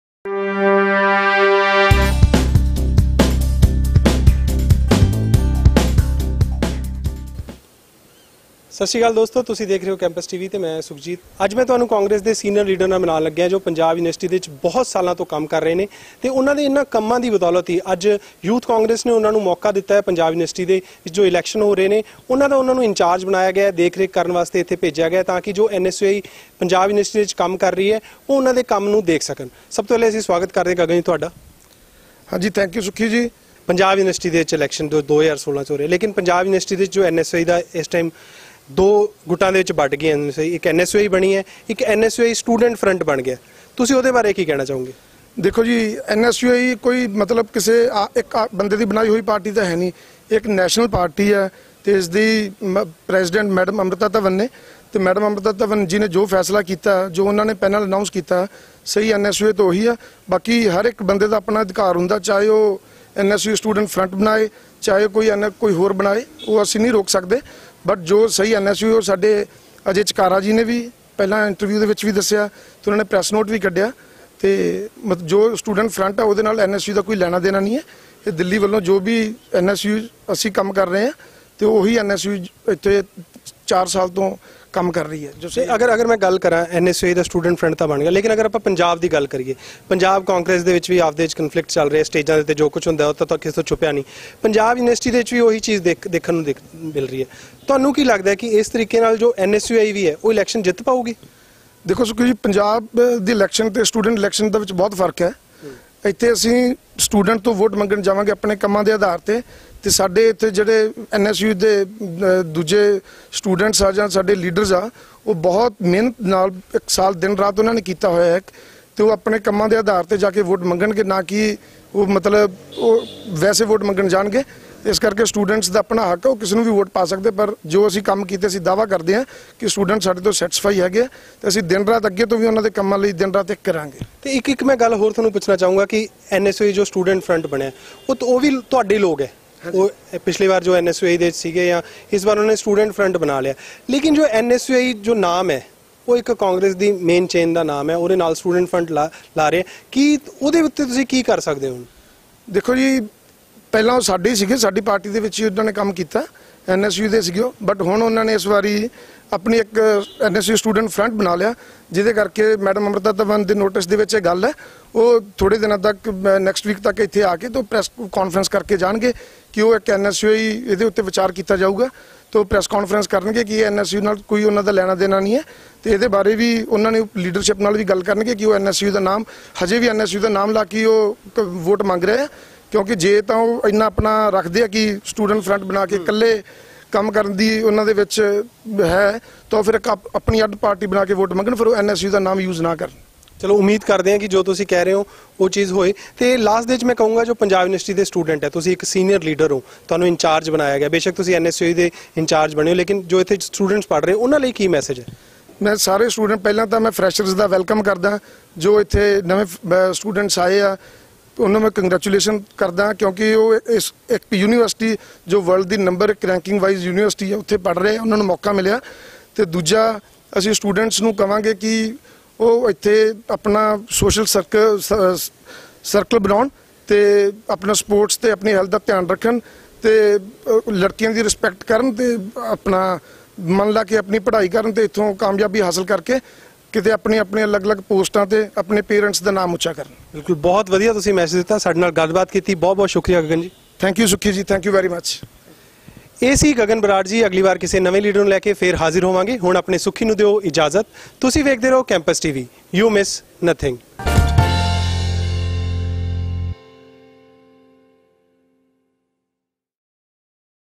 Exclusive Interview | Gagan Brar | Senior Leader | Indian Youth Congress
Mr. Gagan Brar  appointmented as Election Co-ordination In-charge Youth Congress for PUCSC Elections 2016.